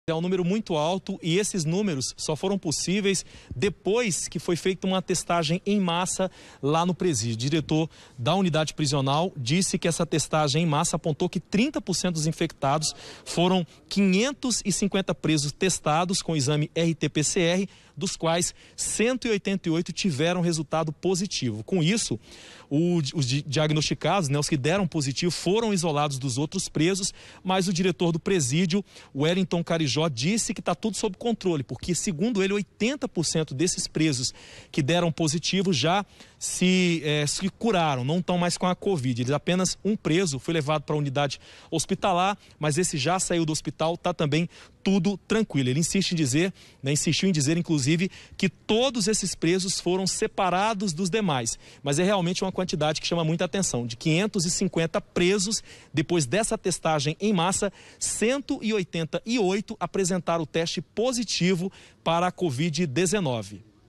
COVID-19: EM RIO VERDE, 30% DOS DETENTOS ESTÃO CONTAMINADOS
TRINTA POR CENTO DOS DETENTOS DO PRESÍDIO DE RIO VERDE ESTÃO COM COVID-19.